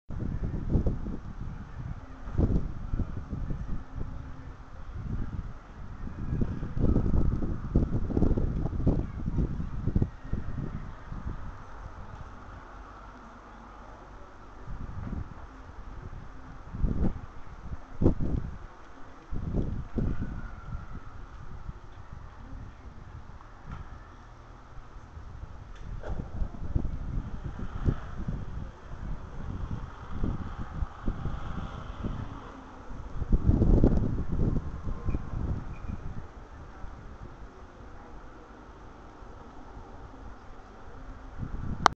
Cut CD wind turbine
Vertical axis Wind Turbine. Double divided CD